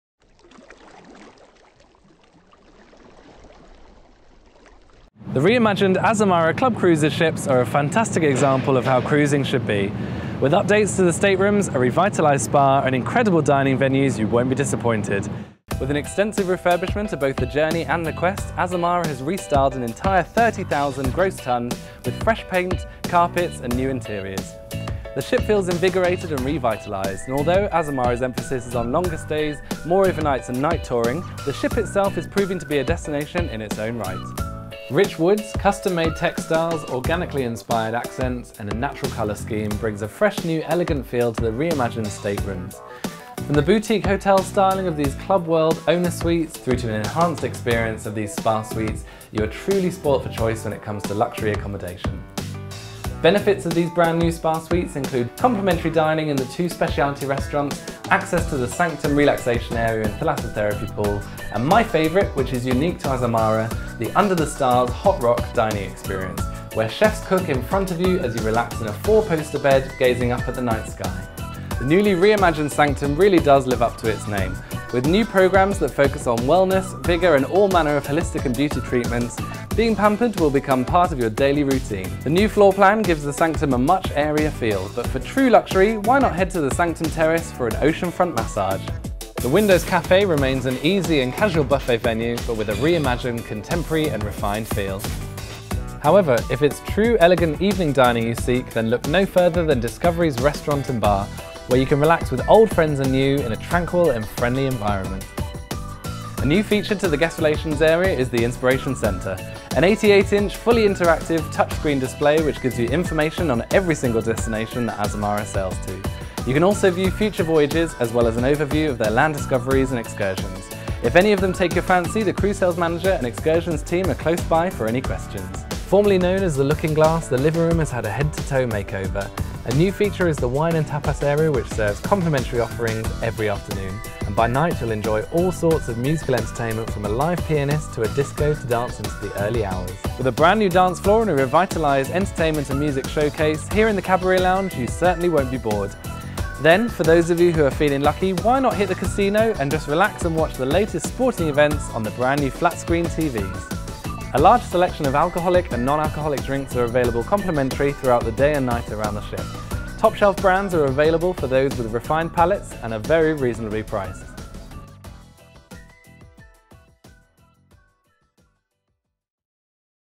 Onboard Azamara Quest Reimagined, Azamara Club Cruises | Planet Cruise
Planet Cruise were recently invited onboard the newly Reimagined Azamara Club Cruises ships. Join Chris as he guides you through the newly Reimagined Azamara Quest.